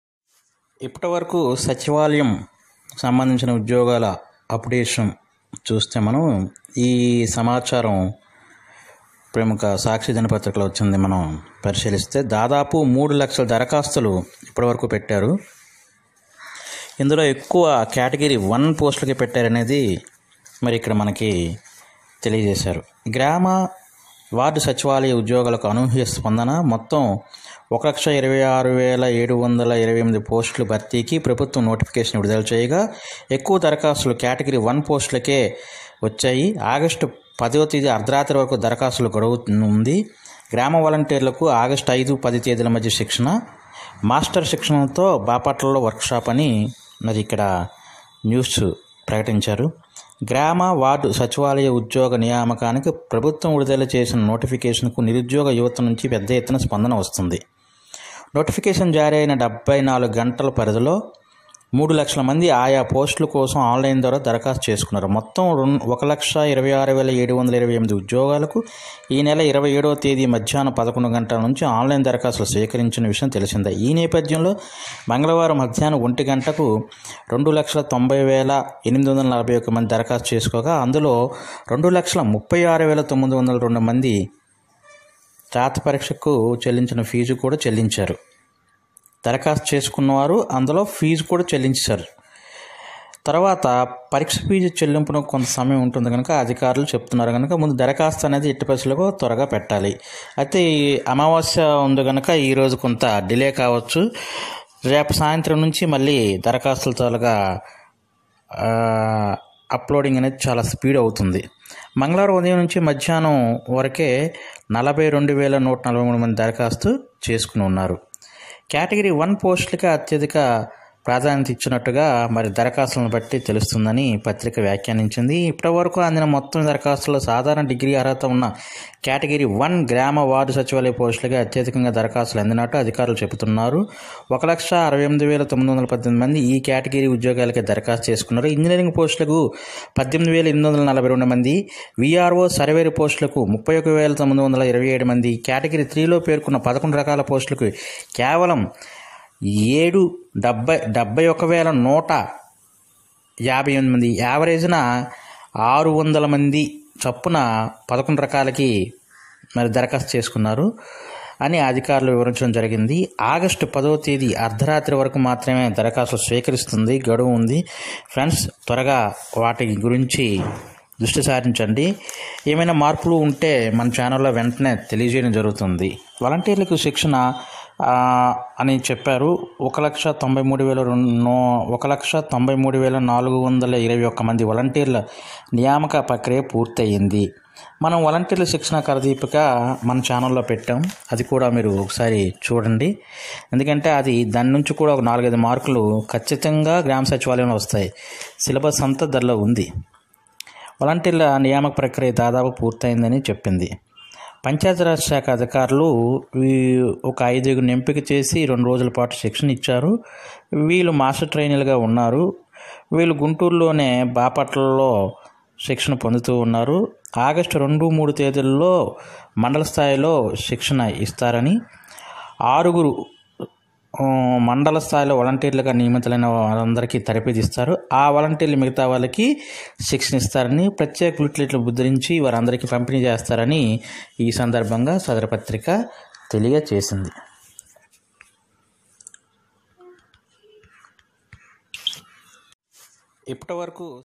3 లక్షలు దాటిన దరఖాస్తులు.గామ సచివాలయం ఉద్యోగాల updates
గ్రామ వలంటీర్స్ ట్రైనింగ్ వివరాలు, గామ సచివాలయం ఉద్యోగాల updates